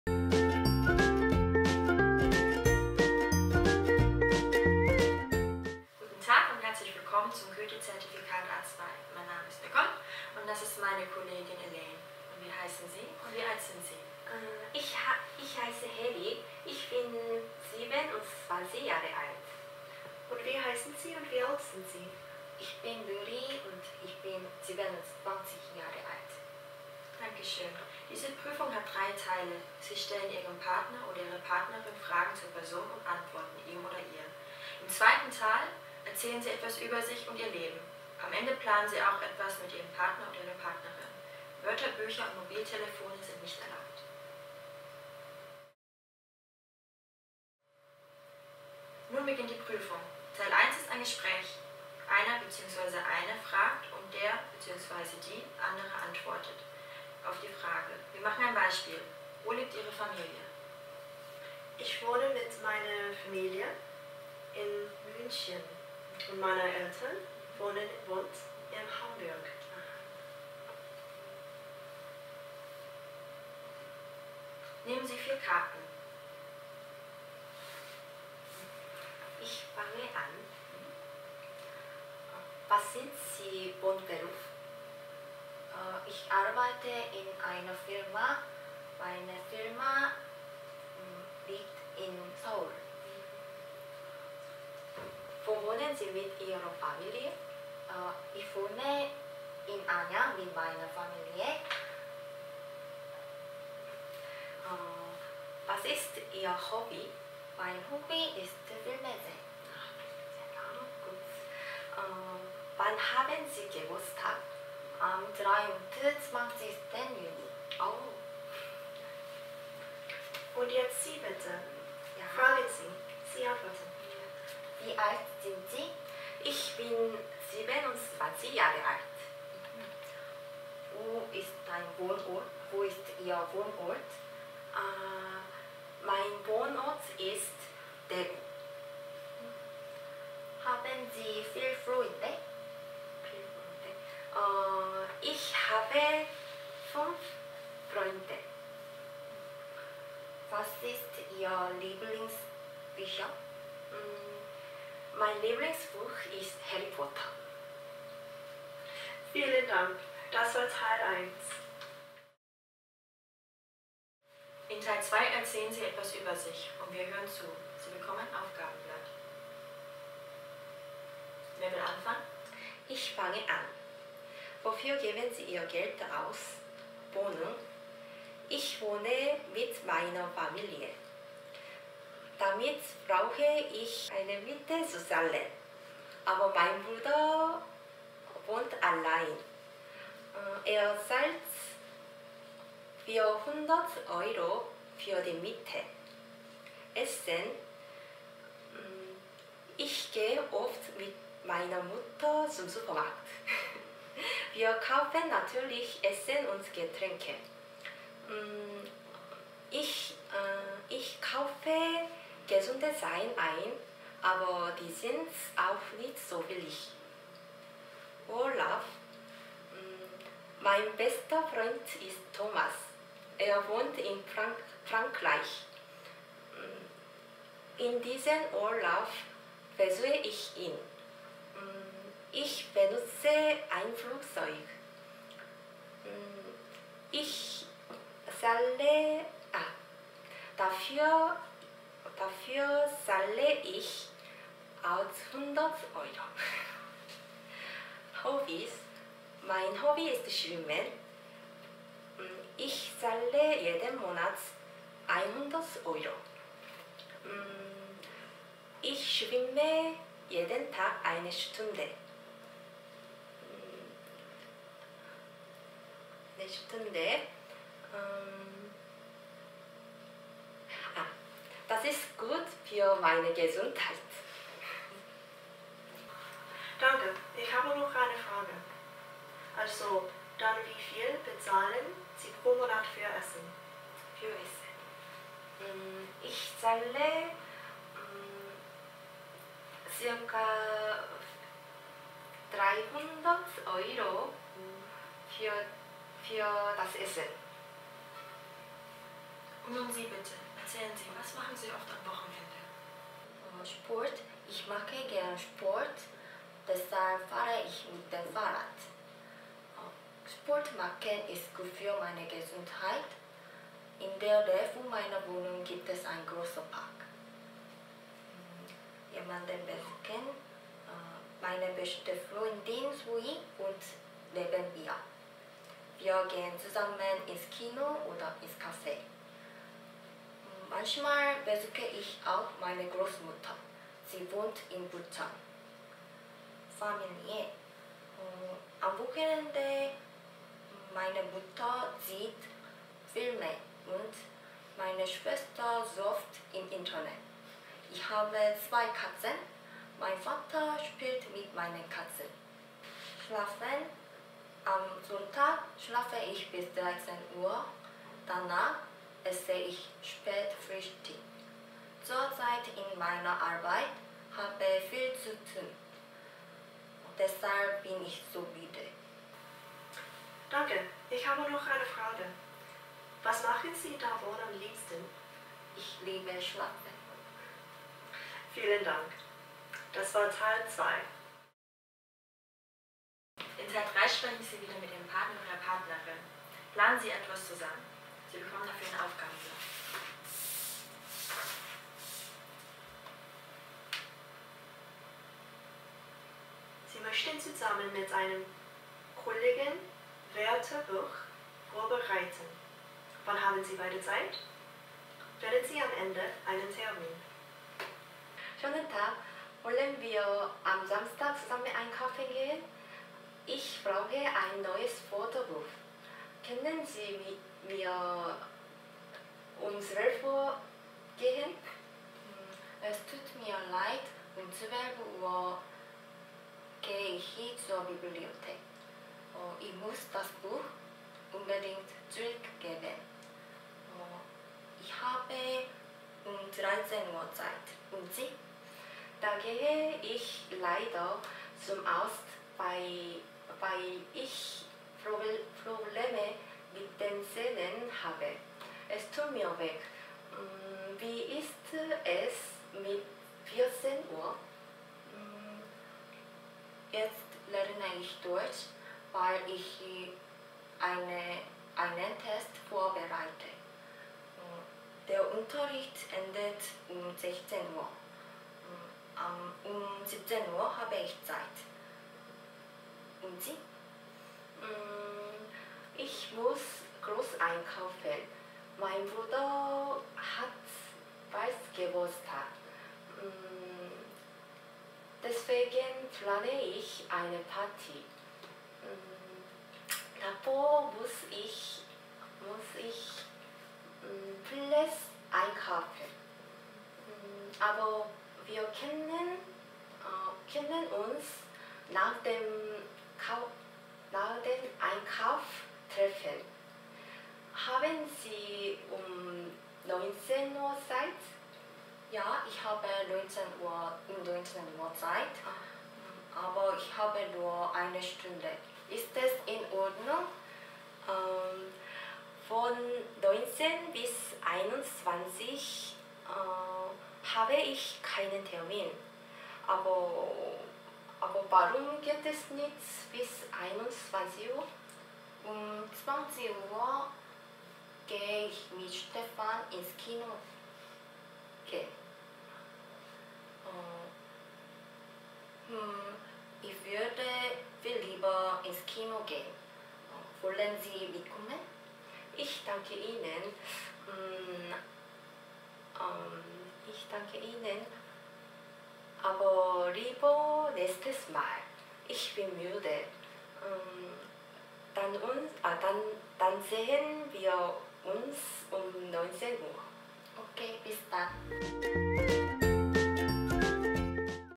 독일어능력시험 Zertifikat Deutsch A2 말하기 모의 시험
독일어능력시험 A2 말하기(Sprechen) 모의 시험 영상입니다.
Sprechen 파트가 어떠한 방식으로 진행되는지 참고하시고,
나라면 어떻게 질문하고 답변했을지 생각하면서 시청하세요.

동양북스 독일어능력시험 A2 도서 자세히 보기